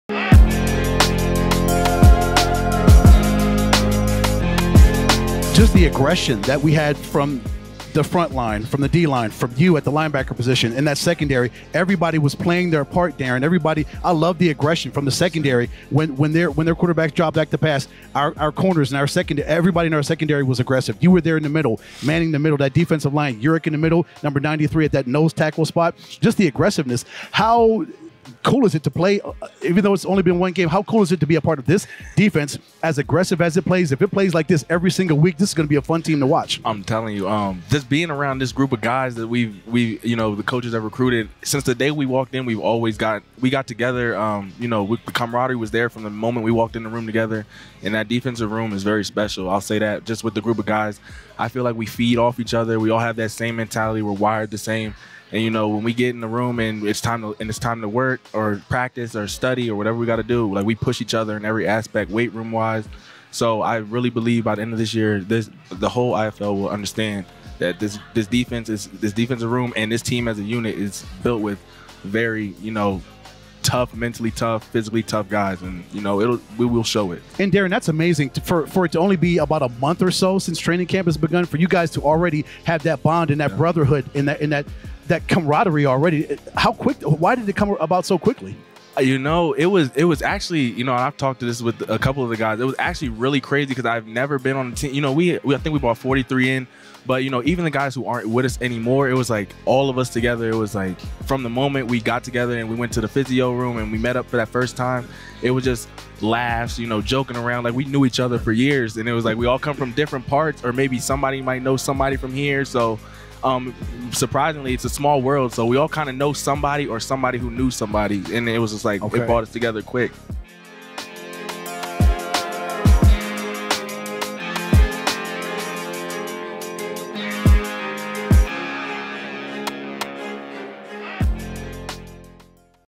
NAZ Wranglers LB Darin Hungerford discusses team brotherhood
Our broadcast team visits with LB Darin Hungerford during the Wranglers Roundup to discuss the brotherhood being formed within the squad!